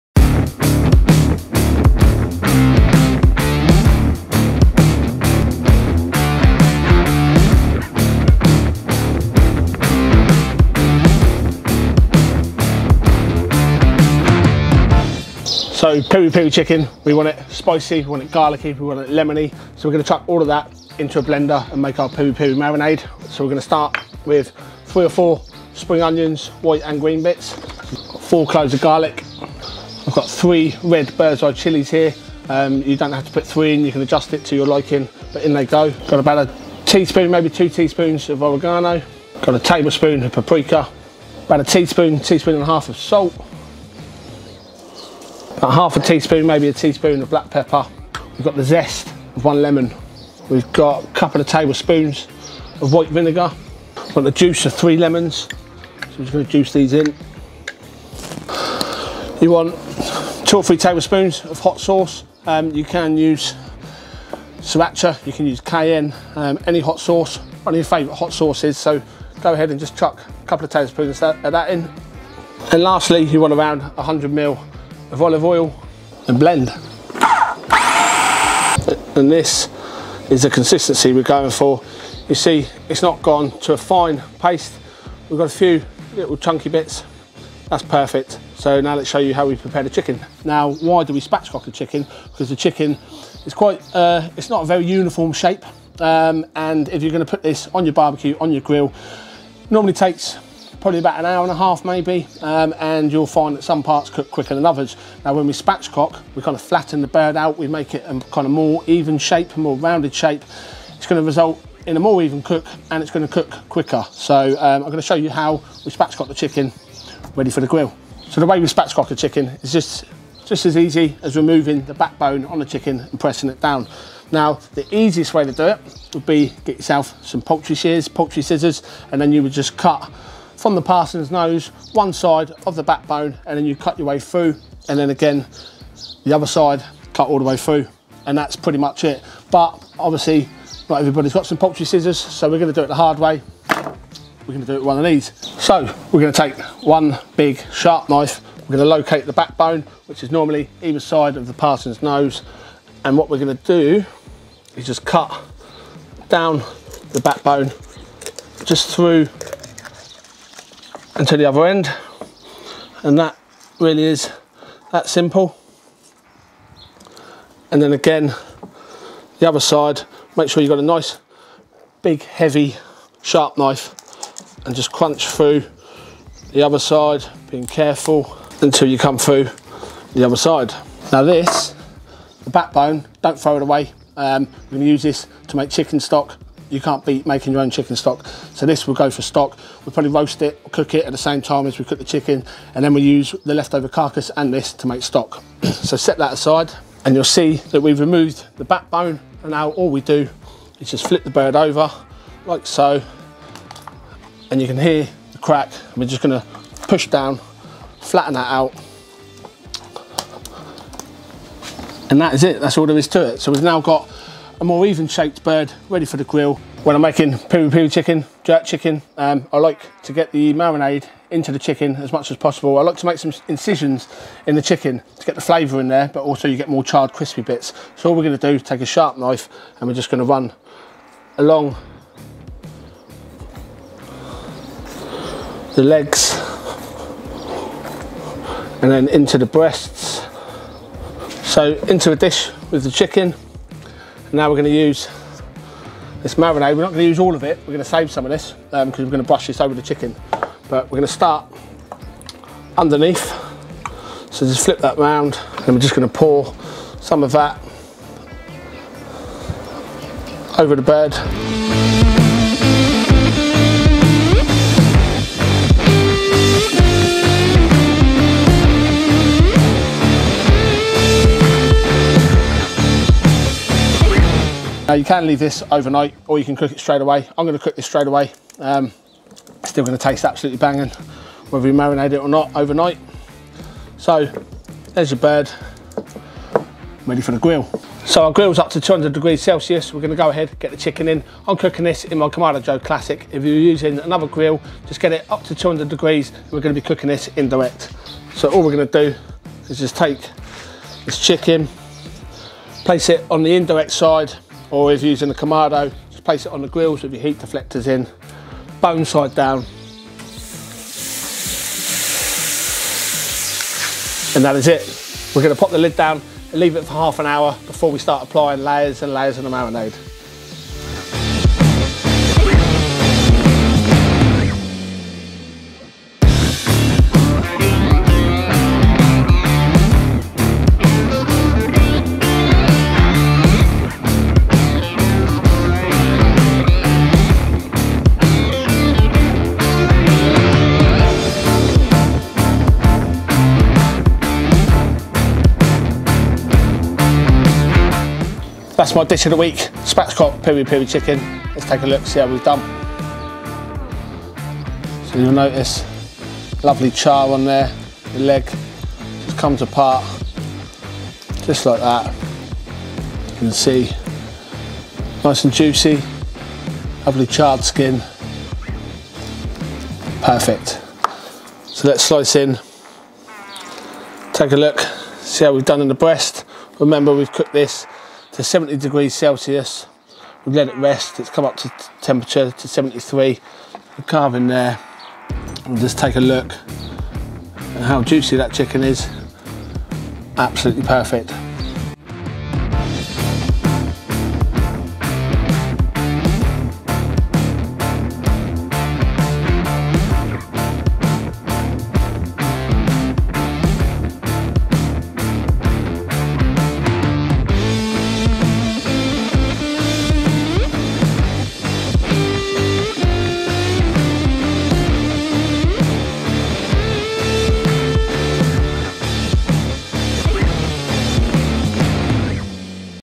Spatchcocked Peri Peri Chicken | Cooked on the Kamado Joe Classic
Here's my spatchcocked peri peri chicken, cooked on the Kamado Joe Classic 3. Spatchcocking a chicken helps it cook quicker and more evenly.

Please get in touch if you have any questions or feedback - I'll always try to answer all comments and feedback

If you've enjoyed this video then please check me out on other social media platforms below...